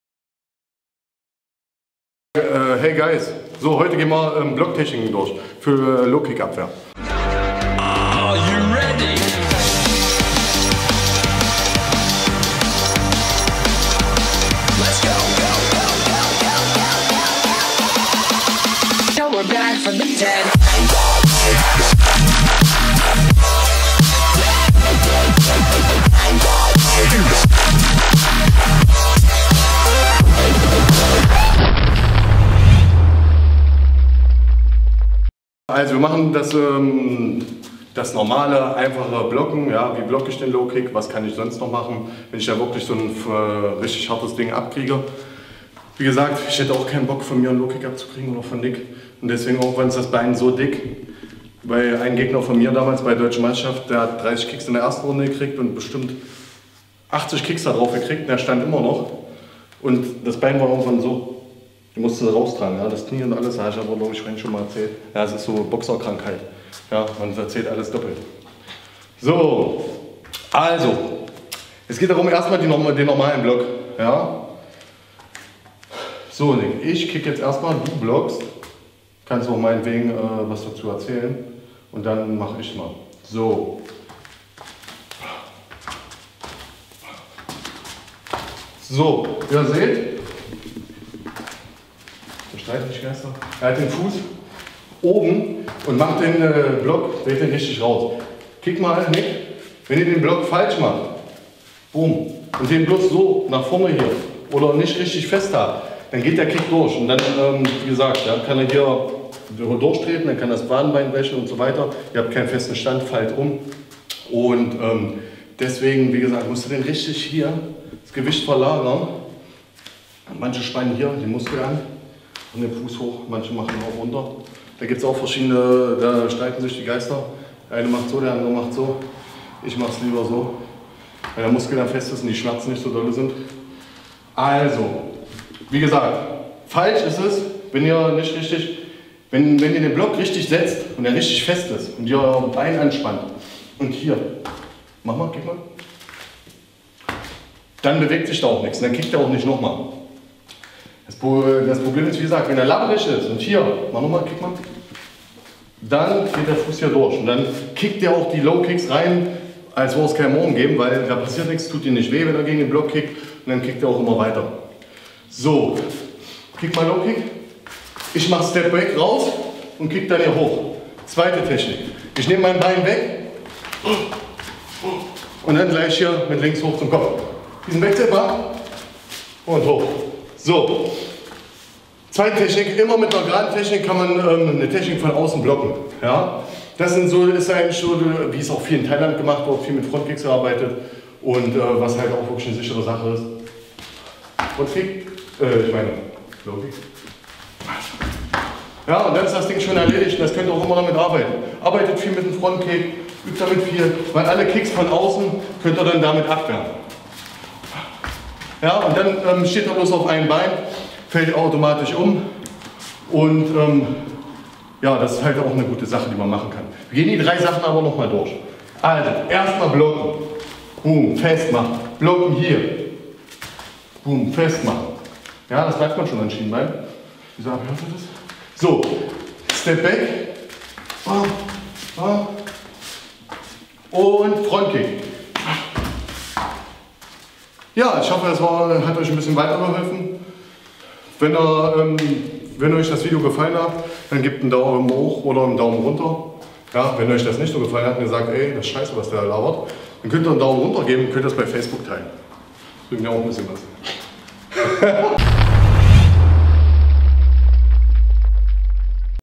Techniken zum Blocken
Heute zeige ich Euch wie ihr Tritte  blocken könnt.